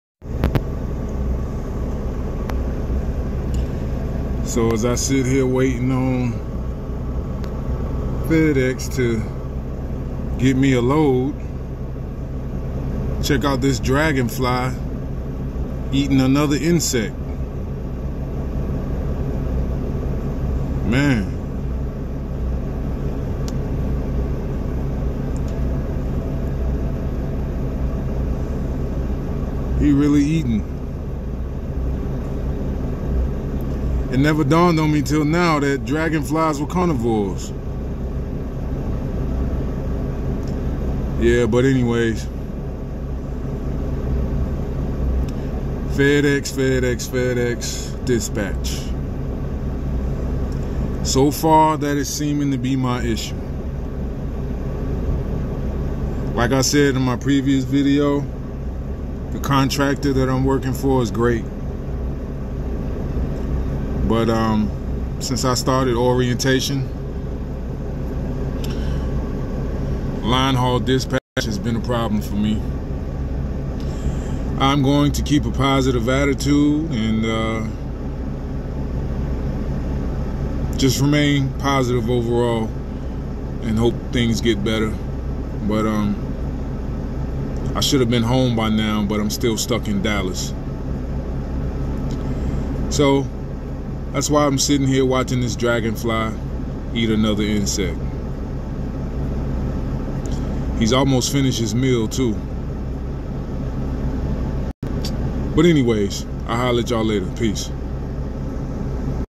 FedEx Ground Linehaul Dispatch - What the Fu@!?
As a new linehaul FedEx ground driver, I am learning how horrible the wait times can be to get a load! Frustrating!